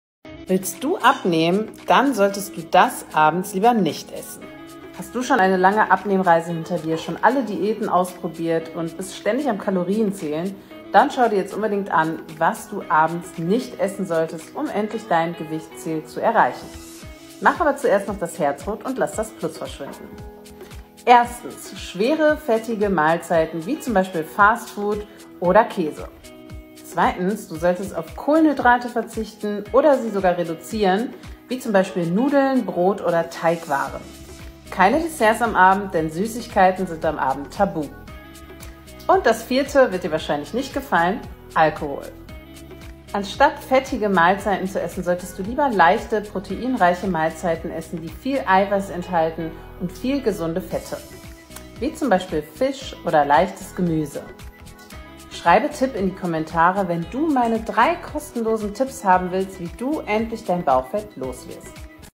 Iss das Abends nicht wenn du abnehmen willst!! schreibe Tipp in die Kommentare, wenn du meine 3 kost
Iss das Abends nicht wenn du abnehmen willst!! schreibe Tipp in die Kommentare, wenn du meine 3 kostenlosen Tipps haben willst gegen Bauchfett.